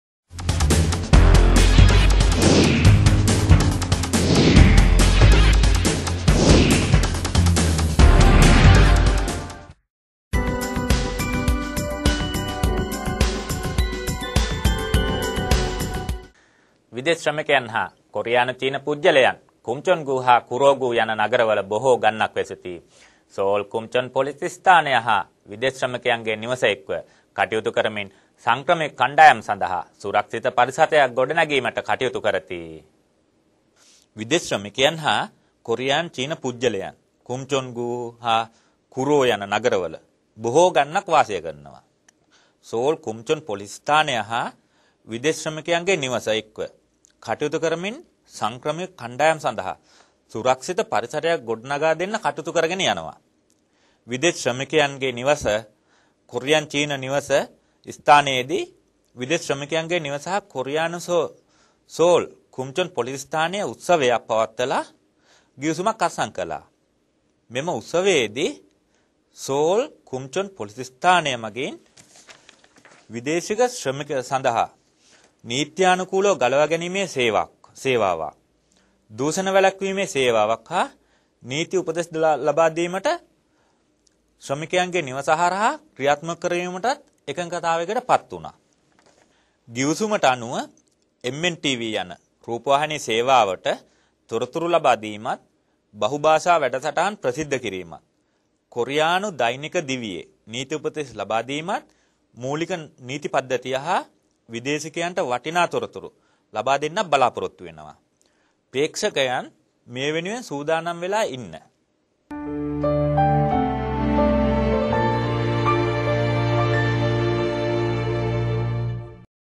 SLK3-1.wmv
[Seoul Geumchun Police Station and Migrant Workers House Reaches Agreement]
Geumchun-gu and Guro-gu in Seoul is highly populated with migrants including the Korean-Chinese community and migrant workers. Seoul Geumchun Police Station and Migrant Workers House is working together to provide a safer living environment for the migrant community.